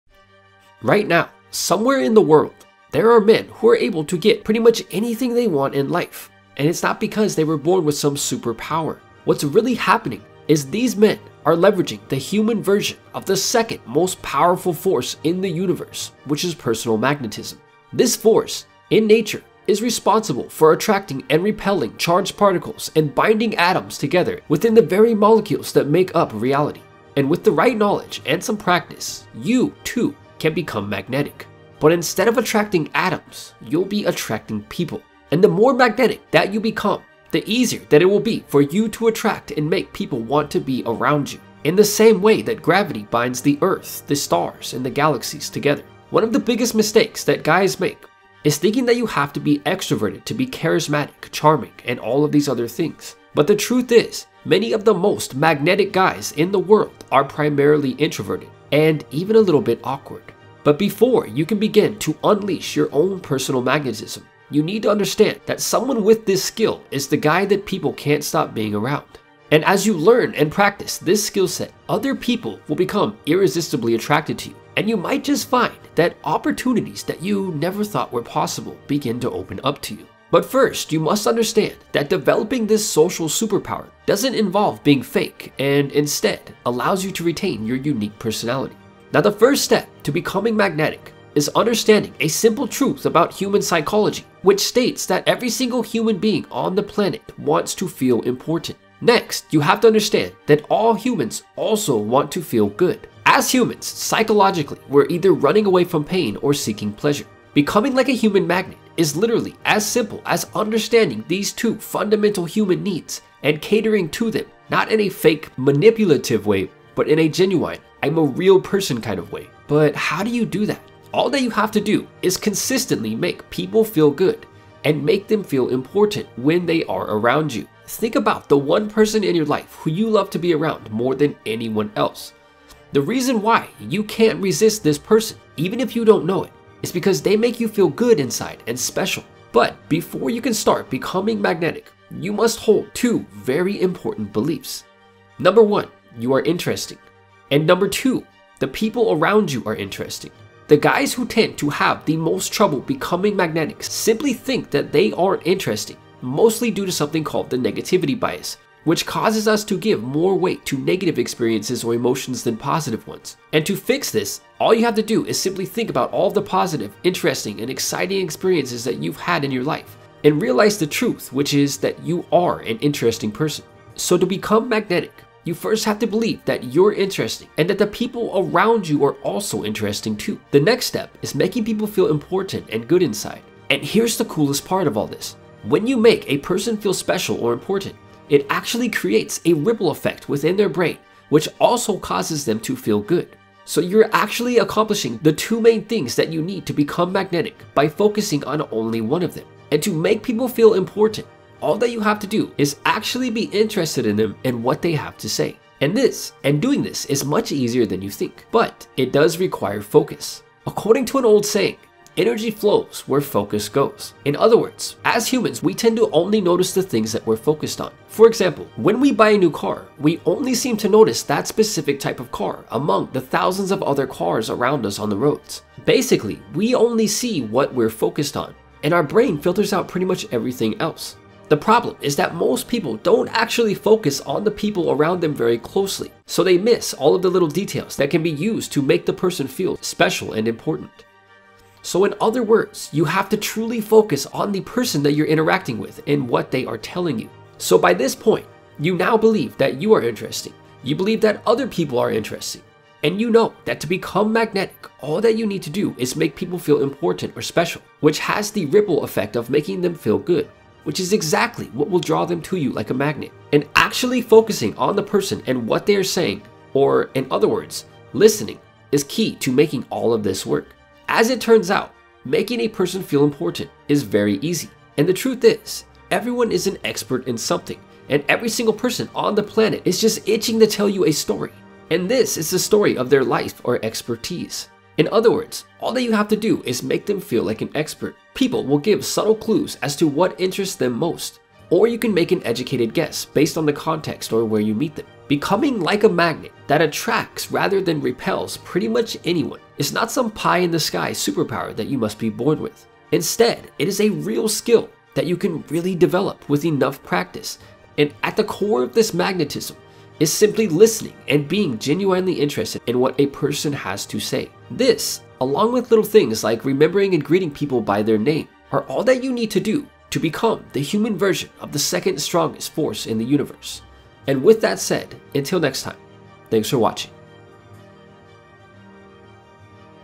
How To Become Magnetic
Find out how to become magnetic and develop a magnetic personality. People skills are extremely useful in this world we live in if you want to achieve success in pretty much anything.